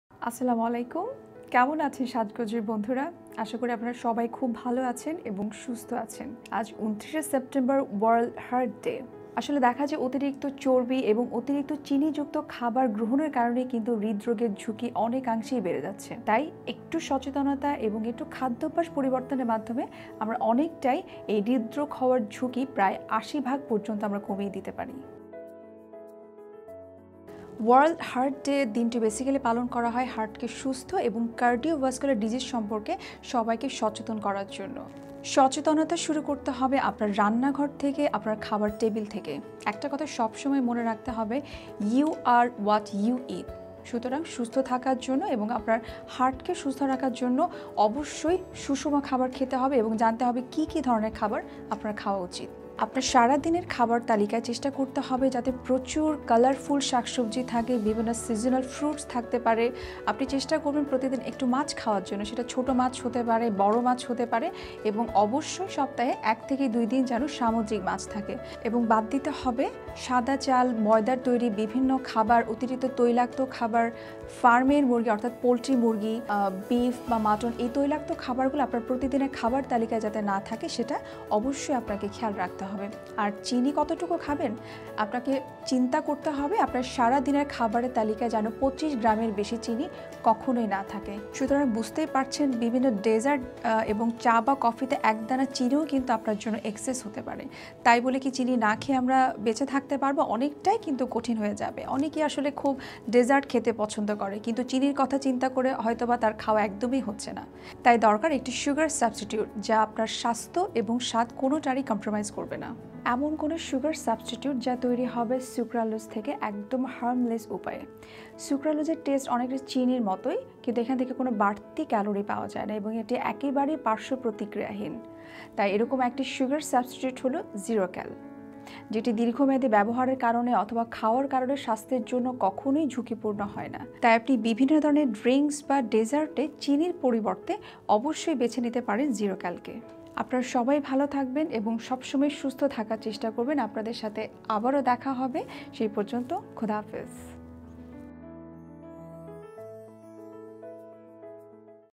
সুস্থ জীবনের জন্য সুস্থ হার্ট | Healthy Heart | Nutritionist Advice
আজ "ওয়ার্ল্ড হার্ট ডে" উপলক্ষে আমরা পুষ্টিবিদ নুসরাত জাহানের কাছ থেকে জানবো আমাদের এই দৈনন্দিন জীবনটাকে সুস্থতার সাথে বাঁচার জন্য হার্ট-কে কীভাবে আমরা সুস্থ রাখতে পারি এবং এর জন্য কী কী করনীয়। চলুন তবে জেনে নেই।

Today on this "World Heart Day", Nutritionist Nusrat Jahan will tell us about how can we keep our heart free from cardiovascular diseases to lead a healthy and happy life. Let's have a look.

ABOUT SHAJGOJ
Shajgoj.com is a destination for bengali speaking women around the world